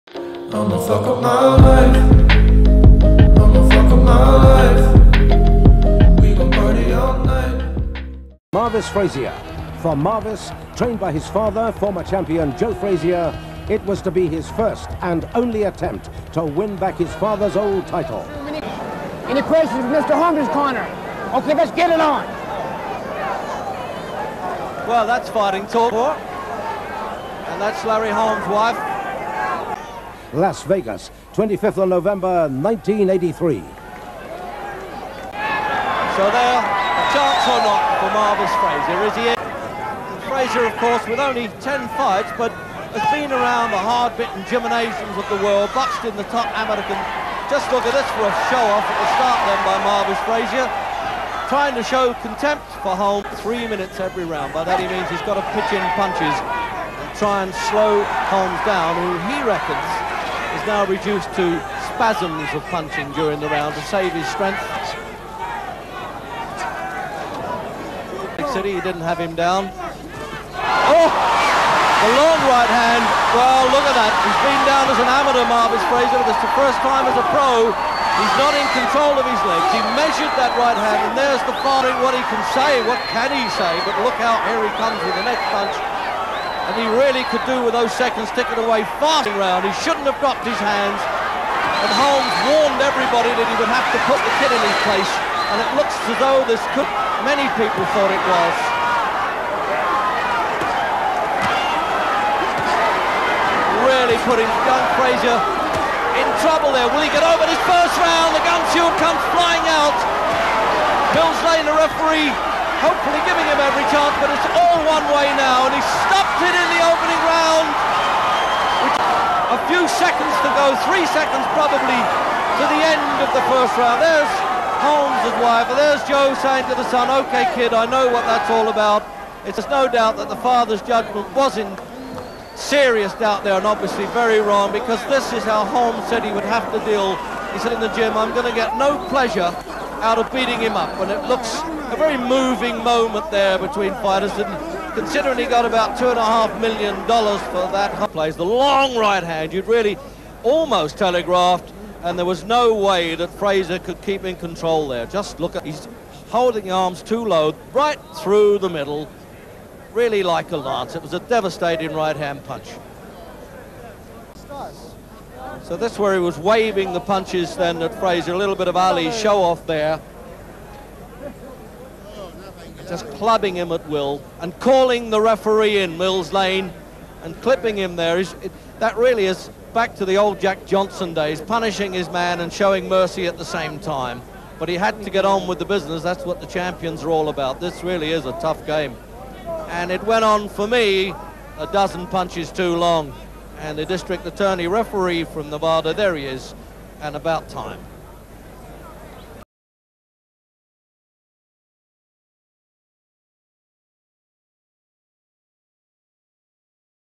Biggest MISMATCH in Boxing History
Larry Holmes (44-0) against Marvis Frazier (10-0)...poor kid!